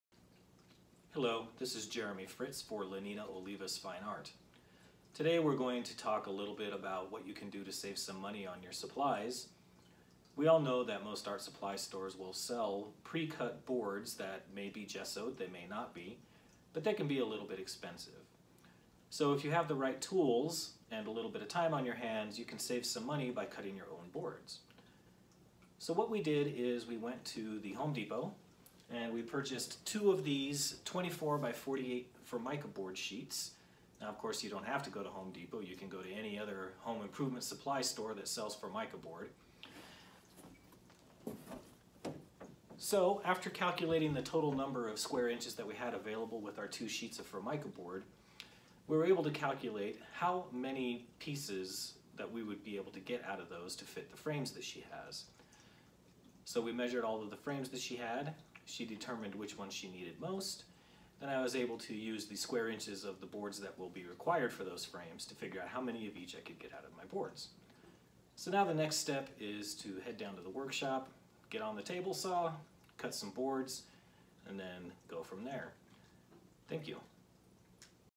How to save money on art supplies
This is a short video on how to save money by cutting your own boards for art supplies.